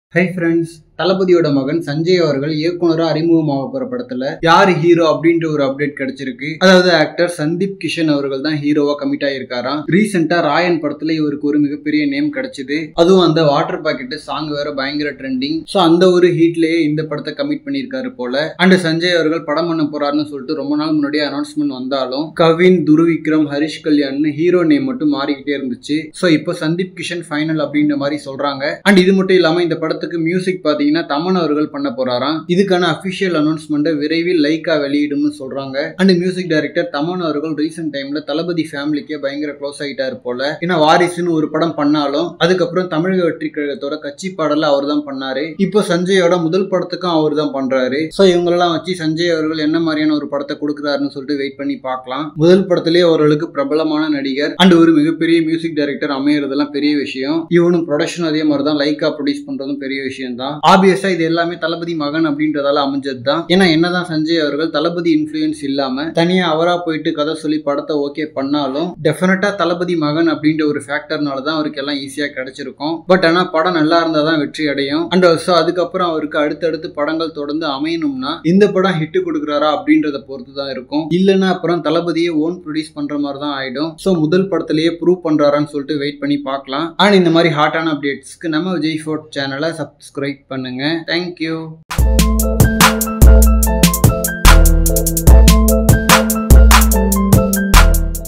விஜய் மகன் படத்தின் ஹீரோவான Sundeep Kishan | Thalapathy Vijay Son Jason Sanjay | The Goat
விஜய் மகன் படத்தின் ஹீரோவான Sundeep Kishan | Thalapathy Vijay Son Jason Sanjay
                  Sundeep Kishan Act as a Lead Hero in Thalapathy Son Jason Sanjay Debut Directional